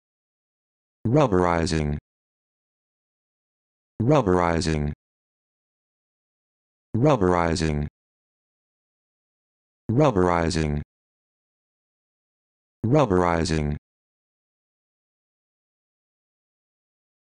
How to Pronounce Rubberizing
This video shows you how to pronounce Rubberizing